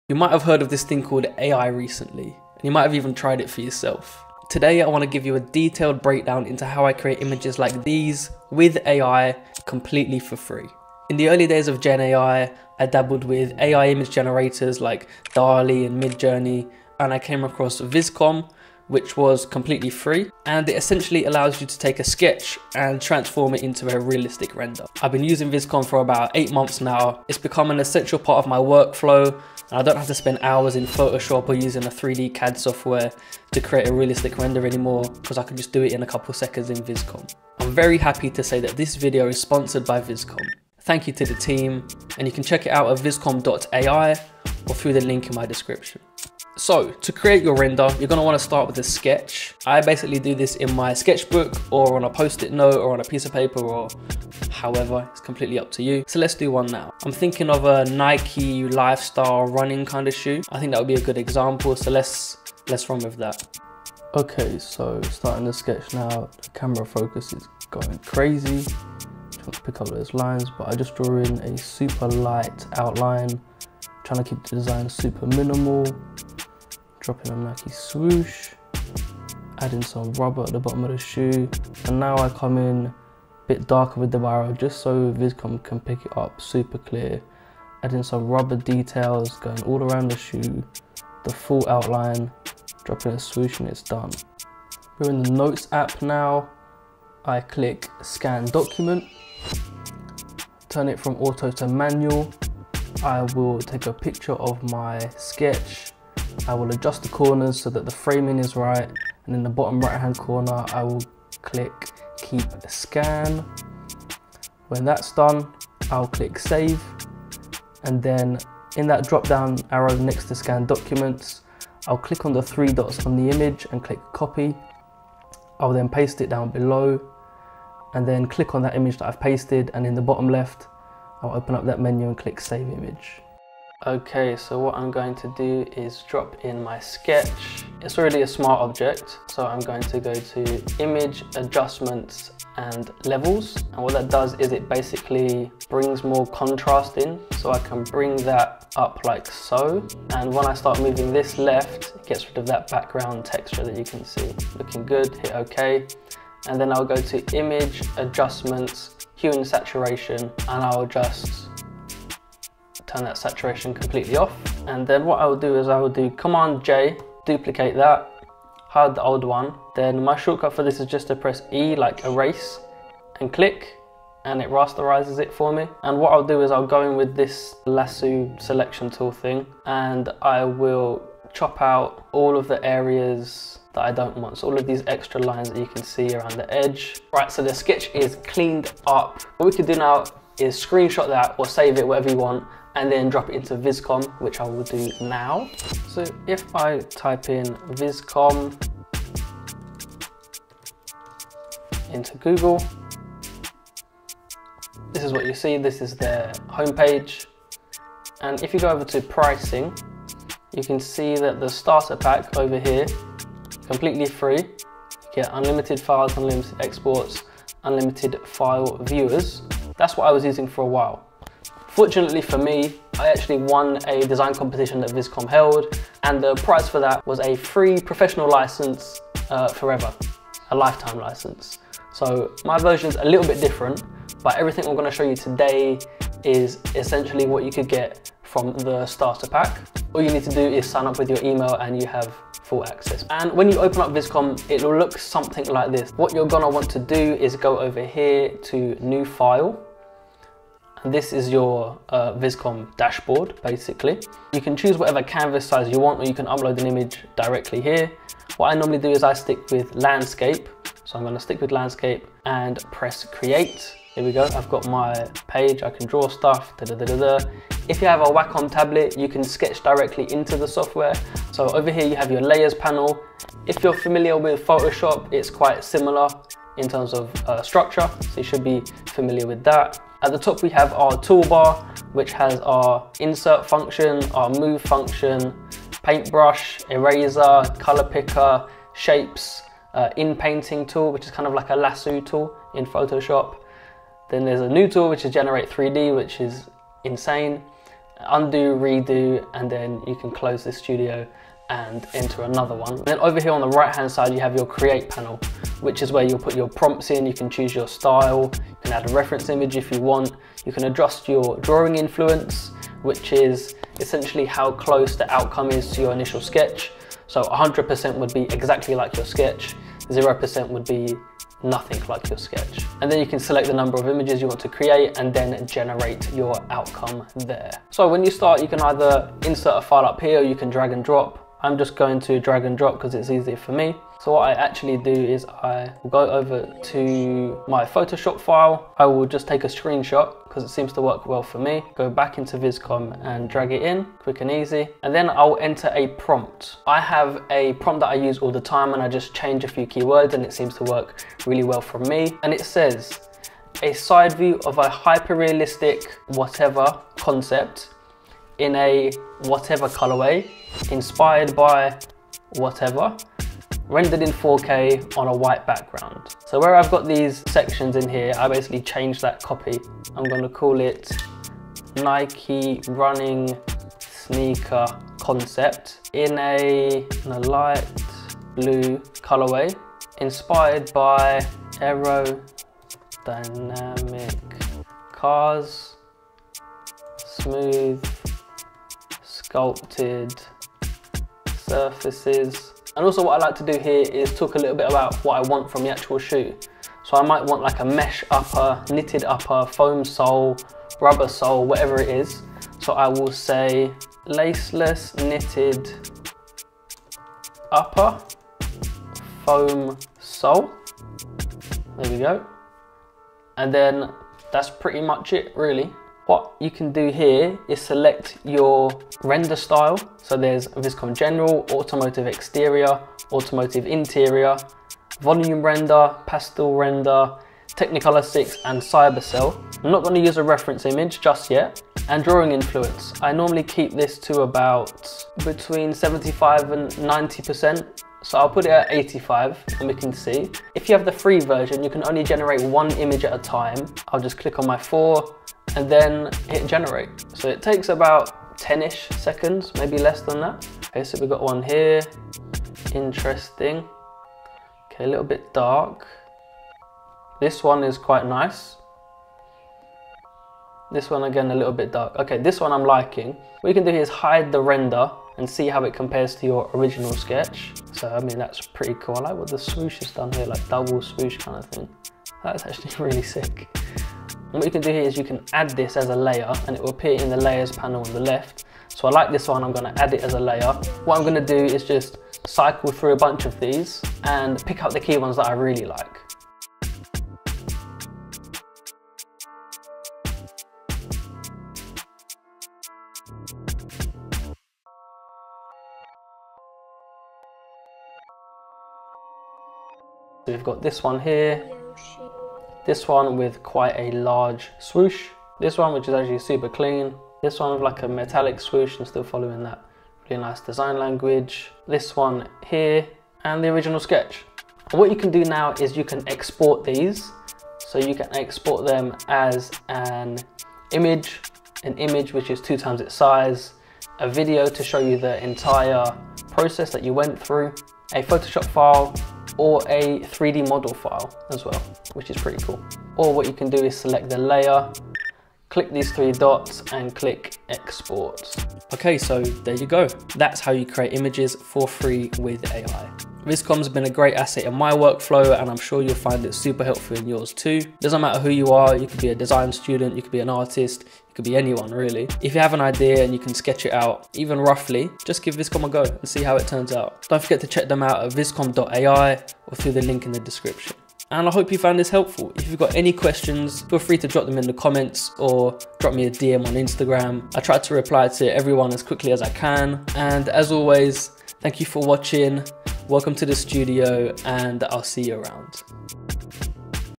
How To Design Sneakers with AI! (Vizcom Tutorial)
Today, we're going to run through how you can creates images for free with AI. I use a tool called Vizcom, and you can find it at:

Chapters:
0:00 Intro
0:46 Brief
1:03 Sketching
1:34 Scanning Sketch
2:12 Photoshop
3:15 Vizcom
8:58 Outro